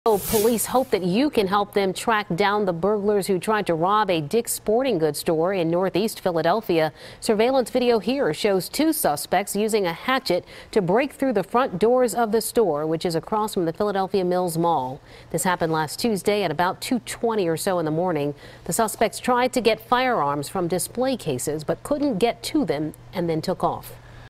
Police Searching For Burglars Who Tried To Rob Dick's Sporting Goods
It happened last Tuesday around 2:20 a.m.